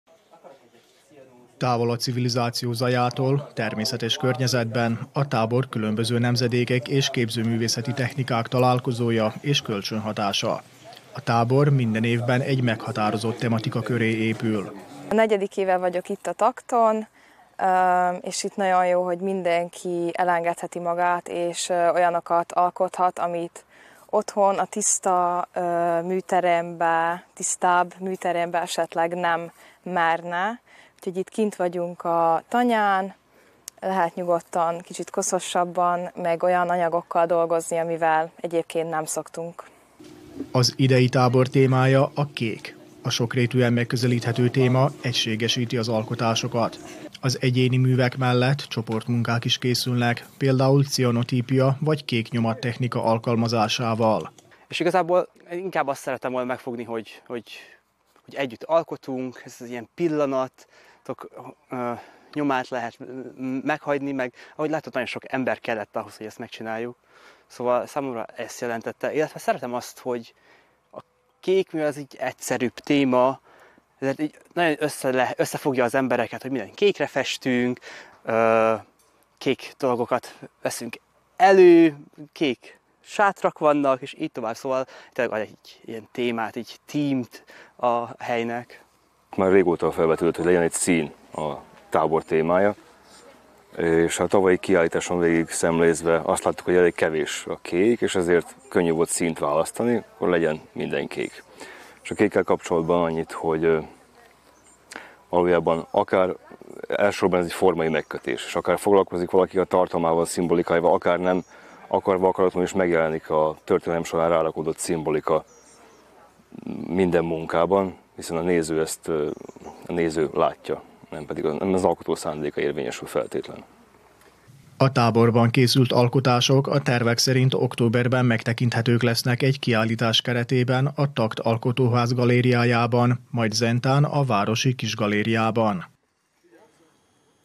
44 TAKT tábor
44. alkalommal szervezték meg a TAKT-ot, a Temerini Alkotóműhelyt és Képzőművészeti Tábort a település közelében lévő Hevér-tanyán. Tíz nap alatt 35 fiatal művész vett részt a táborban.